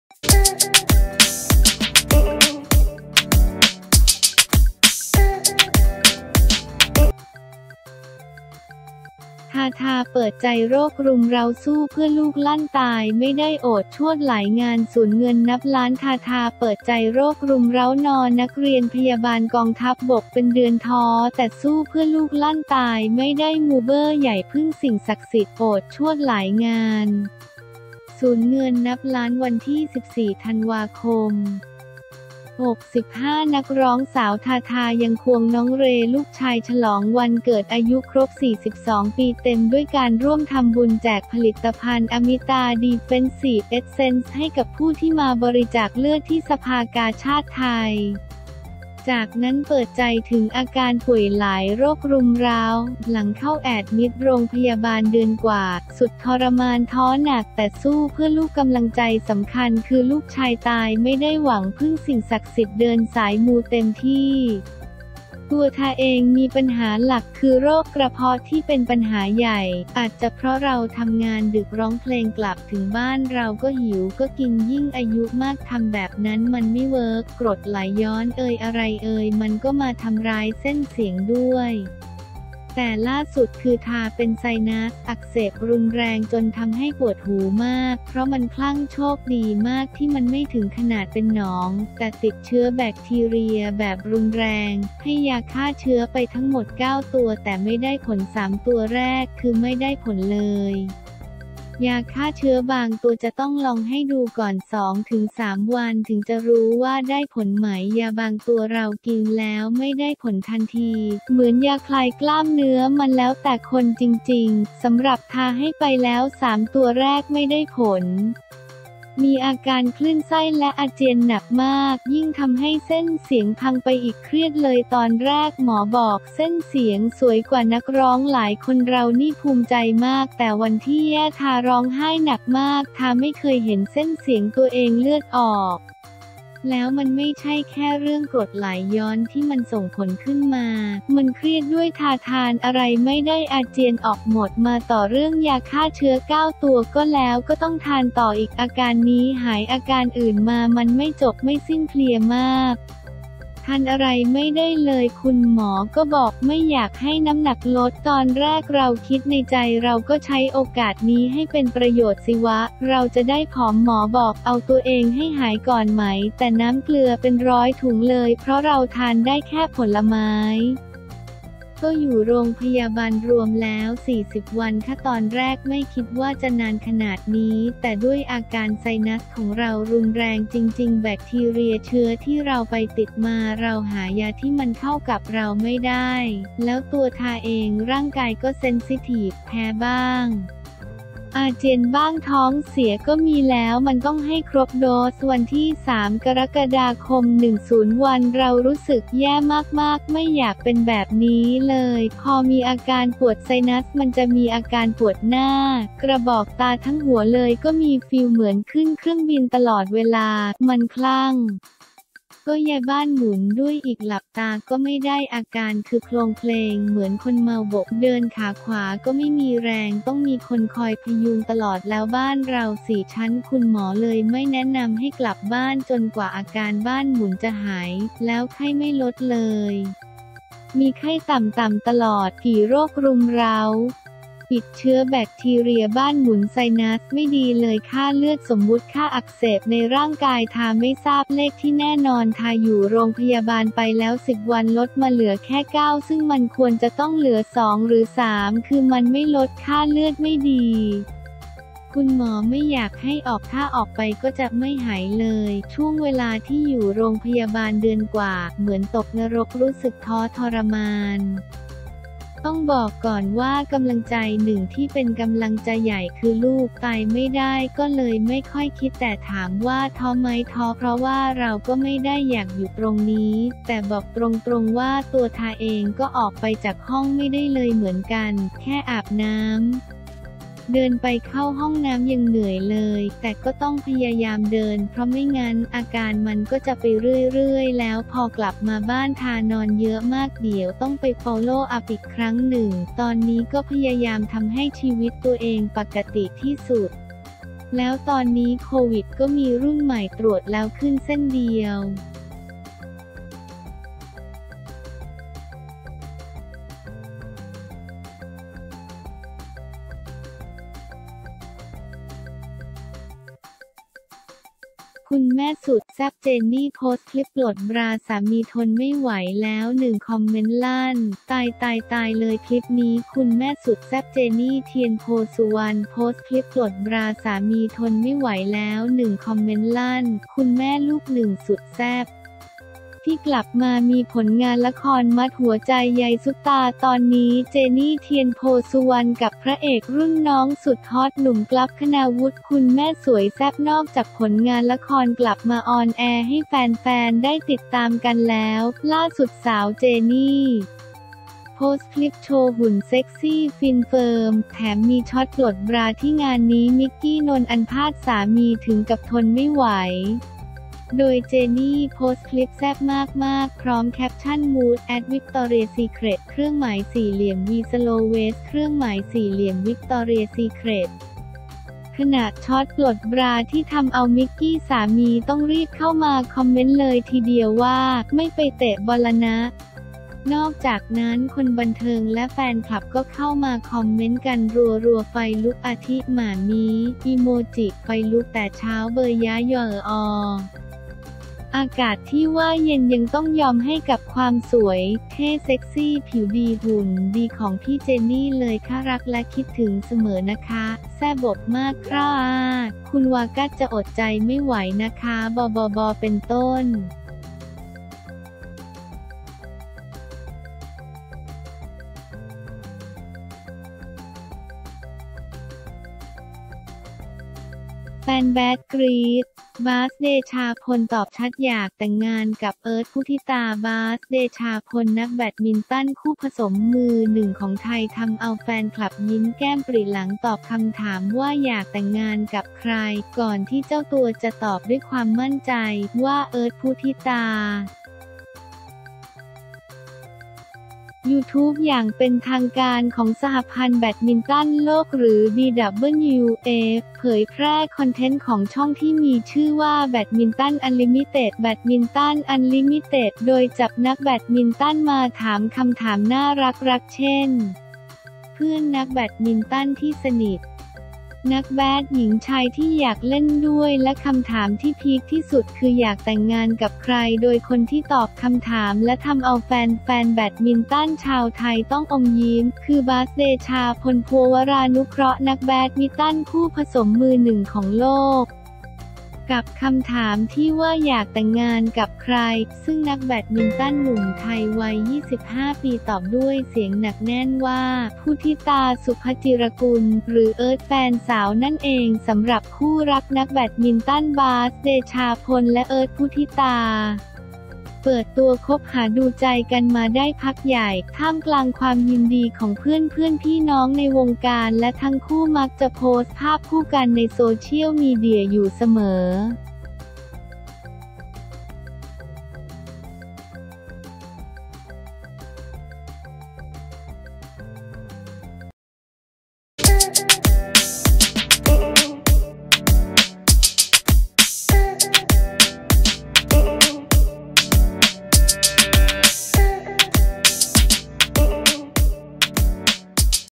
ทาทา เปิดใจ โรครุมเร้า สู้เพื่อลูก ลั่นตายไม่ได้ โอดชวดหลายงาน สูญเงินนับล้าน
ทาทา เปิดใจ โรครุมเร้า นอนรพ.เป็นเดือน ท้อแต่สู้เพื่อลูก ลั่นตายไม่ได้ มูเบอร์ใหญ่พึ่งสิ่งศักดิ์สิทธิ์ โอดชวดหลายงาน สูญเงินนับล้าน
วันที่ 14 ธ.ค. 65 นักร้องสาว ทาทา ยัง ควง น้องเรย์ ลูกชาย ฉลองวันเกิดอายุครบ 42 ปี เต็ม ด้วยการร่วมทำบุญแจกผลิตภัณฑ์ อมิตา ดีเฟนซีฟ เอสเซนส์ ให้กับผู้ที่มาบริจาคเลือด ที่สภากาชาดไทย
จากนั้นเปิดใจถึงอาการป่วยหลายโรครุมเร้า หลังเข้าแอดมิตโรงพยาบาลเดือนกว่า สุดทรมาน ท้อหนักแต่สู้เพื่อลูก กำลังใจสำคัญคือลูกชาย ตายไม่ได้ หวังพึ่งสิ่งศักดิ์สิทธิ์ เดินสายมูเต็มที่
“ตัวทาเองมีปัญหาหลักคือโรคกระเพาะที่เป็นปัญหาใหญ่ อาจจะเพราะเราทำงานดึก ร้องเพลงกลับถึงบ้านเราก็หิว ก็กิน ยิ่งอายุมากทำแบบนั้นมันไม่เวิร์ก กรดไหลย้อนเอยอะไรเอยมันก็มาทำร้ายเส้นเสียงด้วย
แต่ล่าสุดคือทาเป็นไซนัสอักเสบรุนแรงจนทำให้ปวดหูมาก เพราะมันคลั่ง โชคดีมากที่มันไม่ถึงขนาดเป็นหนอง แต่ติดเชื้อแบคทีเรียแบบรุนแรง ให้ยาฆ่าเชื้อไปทั้งหมด 9 ตัว แต่ไม่ได้ผล 3 ตัวแรกคือไม่ได้ผลเลย ยาฆ่าเชื้อบางตัวจะต้องลองให้ดูก่อน 2-3 วันถึงจะรู้ว่าได้ผลไหม ยาบางตัวเรากินแล้วไม่ได้ผลทันที เหมือนยาคลายกล้ามเนื้อ มันแล้วแต่คนจริงๆ”

“สำหรับทาให้ไปแล้ว 3 ตัวแรก ไม่ได้ผล มีอาการคลื่นไส้และอาเจียนหนักมาก ยิ่งทำให้เส้นเสียงพังไปอีก เครียดเลย ตอนแรกหมอบอกเส้นเสียงสวยกว่านักร้องหลายคนเรานี่ภูมิใจมาก แต่วันที่แย่ ทาร้องไห้หนักมาก ทาไม่เคยเห็นเส้นเสียงตัวเองเลือดออก แล้วมันไม่ใช่แค่เรื่องกรดไหลย้อนที่มันส่งผลขึ้นมา มันเครียดด้วย ทาทานอะไรไม่ได้อาเจียนออกหมด”

“มาต่อเรื่องยาฆ่าเชื้อ 9 ตัวก็แล้ว ก็ต้องทานต่ออีก อาการนี้หาย อาการอื่นมา มันไม่จบไม่สิ้น เพลียมาก ทานอะไรไม่ได้เลย คุณหมอก็บอกไม่อยากให้น้ำหนักลด ตอนแรกเราคิดในใจเราก็ใช้โอกาสนี้ให้เป็นประโยชน์สิวะ เราจะได้ผอม หมอบอกเอาตัวเองให้หายก่อนไหม แต่น้ำเกลือเป็นร้อยถุงเลย เพราะเราทานได้แค่ผลไม้”
“ก็อยู่โรงพยาบาลรวมแล้ว 40 วันค่ะ ตอนแรกไม่คิดว่าจะนานขนาดนี้แต่ด้วยอาการไซนัสของเรารุนแรงจริงๆ แบคทีเรียเชื้อที่เราไปติดมา เราหายาที่มันเข้ากับเราไม่ได้ แล้วตัวทาเองร่างกายก็เซ้นซิทีฟ แพ้บ้าง อาเจียนบ้าง ท้องเสียก็มี แล้วมันต้องให้ครบโดส 3-7-10 วัน เรารู้สึกแย่มากๆ ไม่อยากเป็นแบบนี้เลย
“พอมีอาการปวดไซนัสมันจะมีอาการปวดหน้า กระบอกตา ทั้งหัวเลยก็มี ฟีลเหมือนขึ้นเครื่องบินตลอดเวลา มันคลั่ง ก็แย่ บ้านหมุนด้วยอีก หลับตาก็ไม่ได้ อาการคือโคลงเคลงเหมือนคนเมาบก เดินขาขวาก็ไม่มีแรง ต้องมีคนคอยพยุงตลอด แล้วบ้านเรา 4 ชั้น คุณหมอเลยไม่แนะนำให้กลับบ้านจนกว่าอาการบ้านหมุนจะหาย แล้วไข้ไม่ลดเลย มีไข้ต่ำๆตลอด”

กี่โรครุมเร้า? “ติดเชื้อแบคทีเรีย บ้านหมุน ไซนัส ไม่ดีเลย ค่าเลือด สมมติค่าอักเสบในร่างกาย ทาไม่ทราบเลขที่แน่นอน ทาอยู่โรงพยาบาลไปแล้ว 10 วันลดมาเหลือแค่ 9 ซึ่งมันควรจะต้องเหลือ 2 หรือ 3 คือมันไม่ลด ค่าเลือดไม่ดี คุณหมอไม่อยากให้ออก ถ้าออกไปก็จะไม่หายเลย”

ช่วงเวลาที่อยู่โรงพยาบาลเดือนกว่า เหมือนตกนรก รู้สึกท้อทรมาน? “ต้องบอกก่อนว่ากำลังใจหนึ่งที่เป็นกำลังใจใหญ่คือลูก ตายไม่ได้ ก็เลยไม่ค่อยคิด แต่ถามว่าท้อมั้ย ท้อ เพราะว่าเราก็ไม่ได้อยากอยู่ตรงนี้ แต่บอกตรงๆ ว่าตัวทาเองก็ออกไปจากห้องไม่ได้เลยเหมือนกัน แค่อาบน้ำ เดินไปเข้าห้องน้ำยังเหนื่อยเลย
แต่ก็ต้องพยายามเดิน เพราะไม่งั้นอาการมันก็จะไปเรื่อยๆ แล้วพอกลับมาบ้าน ทานอนเยอะมาก เดี๋ยวต้องไปฟอลโลว์อัพอีกครั้งหนึ่ง ตอนนี้ก็พยายามทำให้ชีวิตตัวเองปกติที่สุด แล้วตอนนี้โควิดก็มีรุ่นใหม่ตรวจแล้วขึ้นเส้นเดียว